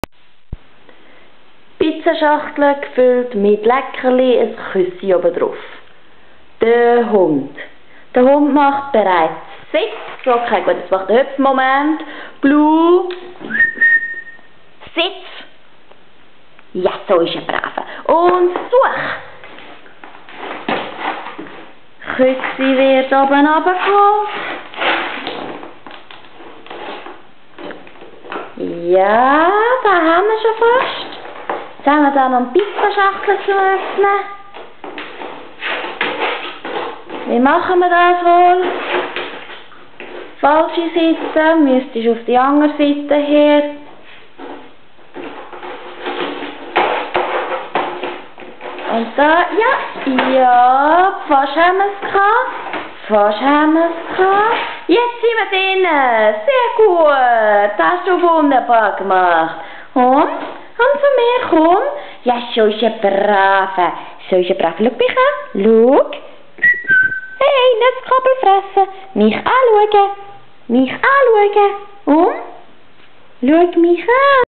Blue - Pizza Schachtel
Er versucht eine Pizza Schachtel zu öffnen, die um es schwieriger zu machen, mit einem Kissen belegt ist. Meisterhaft.